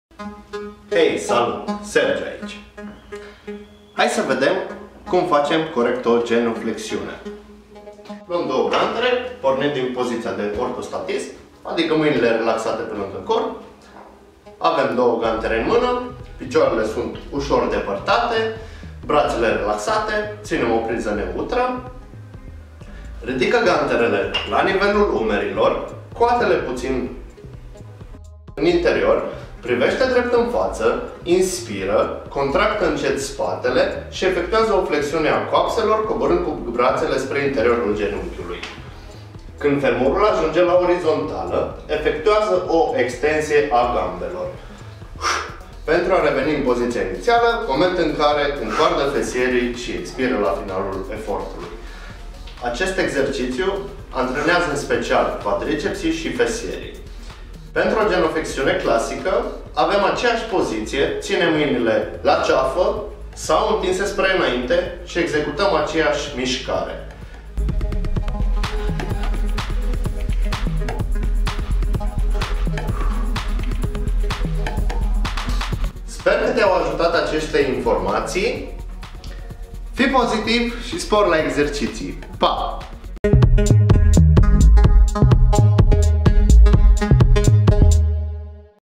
Cum facem corect o Genuflexiune | VIDEO EXPLICATIV
Genuflexiunea (numită și poziție ghemuită, ghemuire, lăsare pe vine, ședere pe vine, îndoire a genunchilor) este o poziție pe care o ia o persoană pentru a se sprijini pe pământ cu genunchii și coapsele îndoite (flectate) și fesele adesea coborâte la nivelul călcâielor. În genuflexiune genunchii sunt îndoiți înainte, coapsele sunt flectate pe trunchi, apropiindu-se de peretele abdominal anterior, iar gambele pe coapse.Hai sa vedem cum executam corect o genuflexiune.